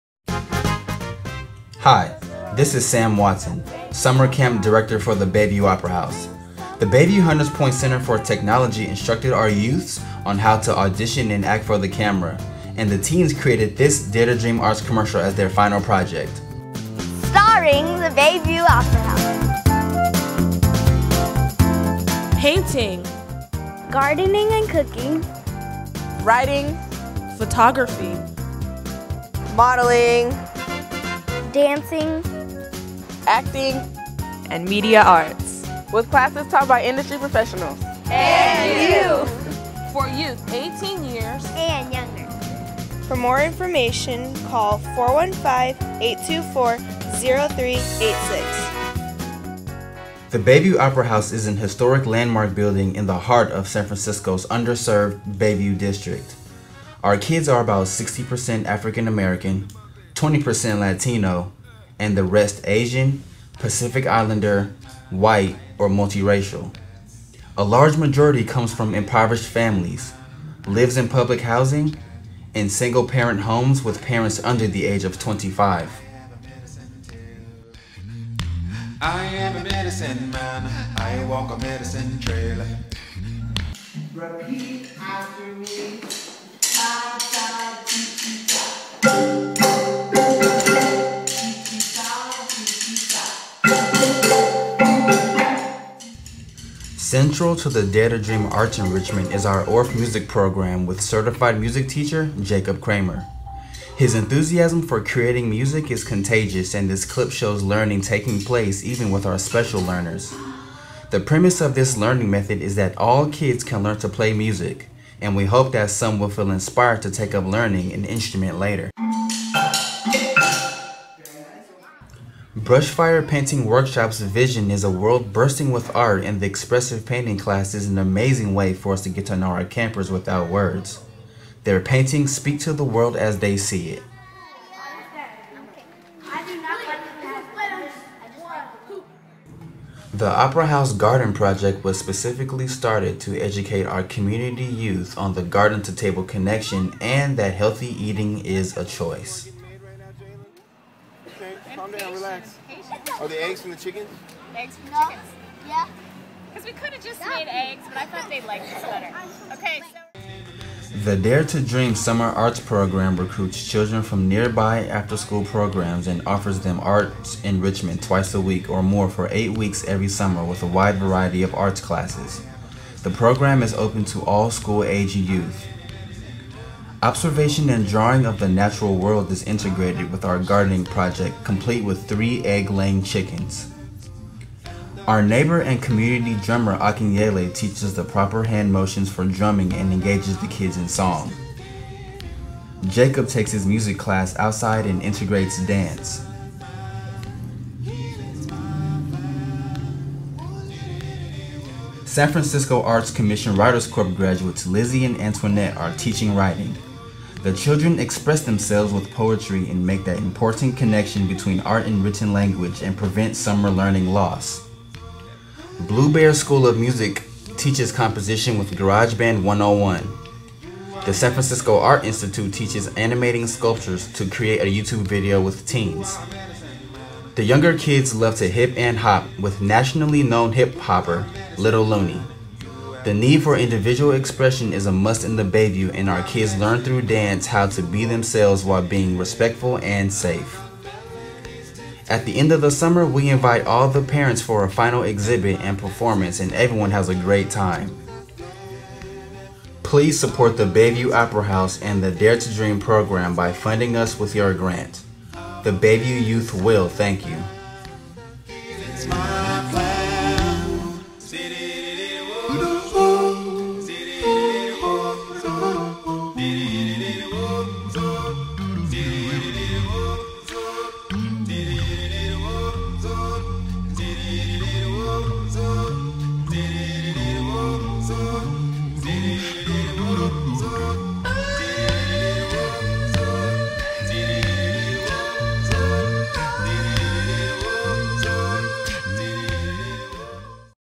Bayview Opera House DARE TO DREAM ARTS Youth Enrichment Programs
The Bayview Opera House is a 501(c)3 organization in San Francisco's Bayview District with a mission to provide arts education to underserved youth. Our 8-week summer program served over 300 low-income children and teens last summer and provides high quality instruction by working artists in many art forms. The program is free to groups from school, child development centers and other non-profit summer programs.
The Bayview Opera House receives basic operational funding from the San Francisco Hotel Tax Fund through the San Francisco Arts Commission and depends on additional grants and private donations to continue and grow our  programs.
The Bayview Opera House is a historic landmark building and was accepted into the National Register of Historic Places on April 1, 2011.